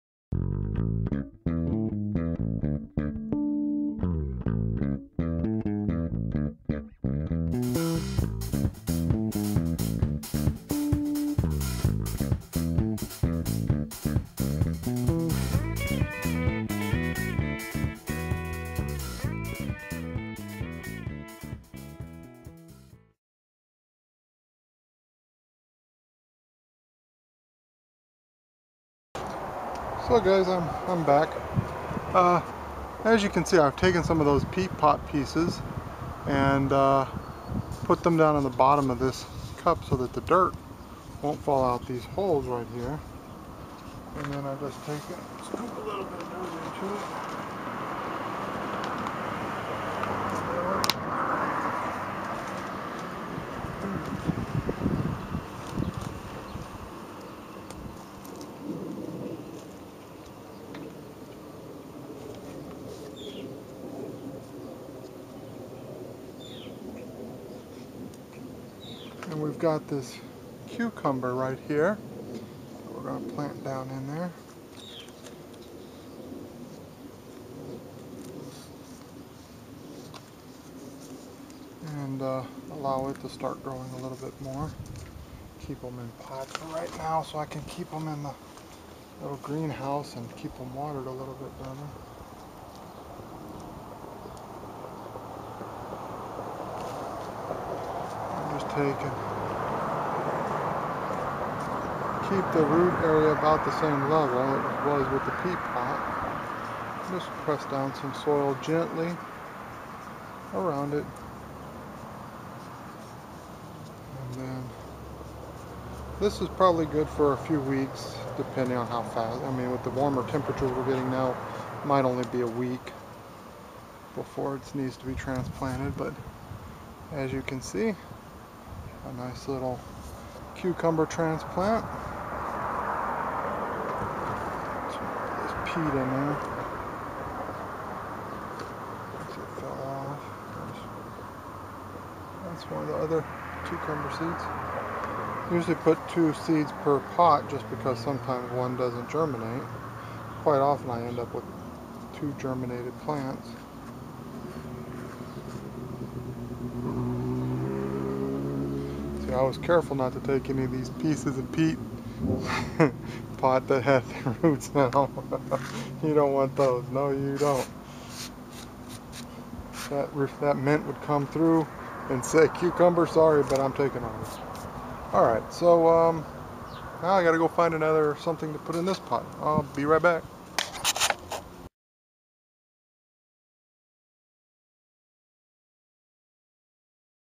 Mark's 2013 Garden - Transplanting a Cucumber Seedling
Mark Demonstrates transplanting a Cucumber Seedling into a pot.